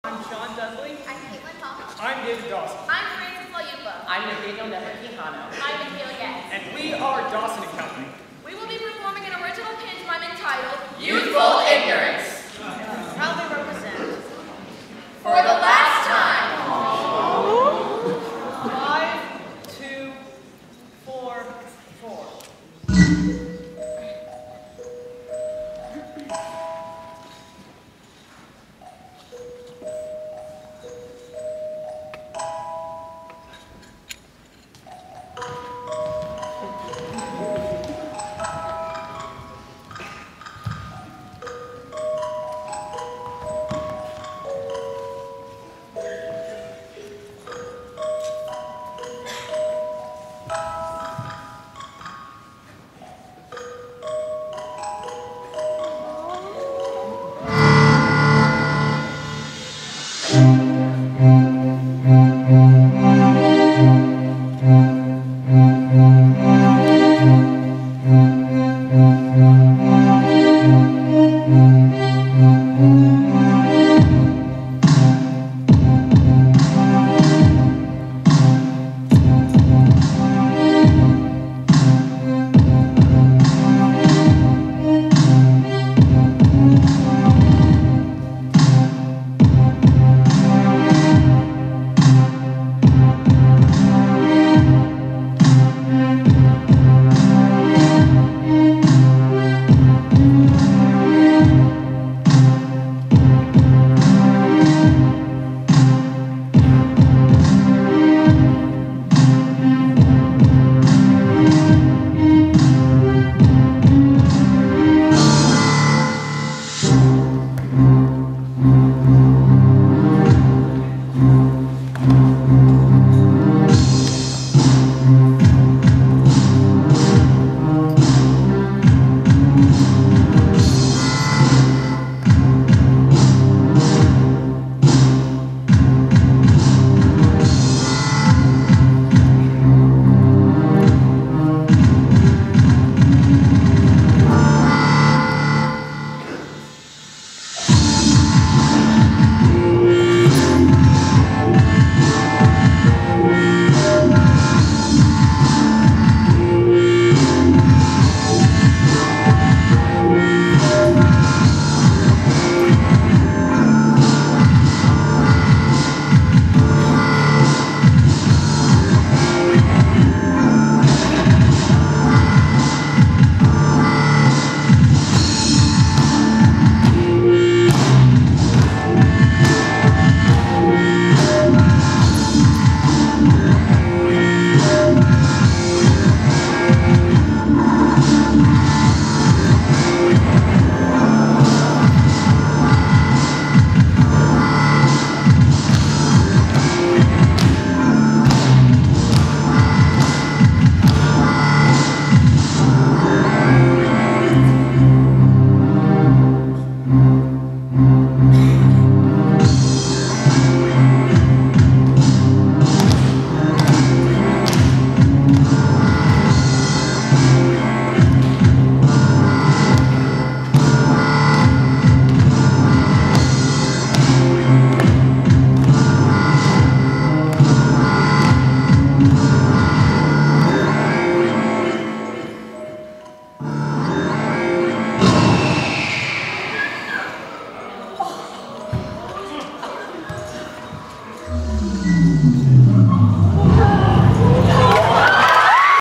Best In Show Pantomime 2013 - "Youthful Ignorance" - Troupe 5244
The 2013 District XI IE Competition's Best In Show selection for Pantomime, entitled "Youthful Ignorance," featuring the talents of John D., Kaitlin H., David D., Frances L., Nathaniel K-Q., & Mikayla G.

-Troupe 5244-

This piece is asterisked for *Prejudice, *Violence, *Religious Content, & *Frank "Pantomime" of Death.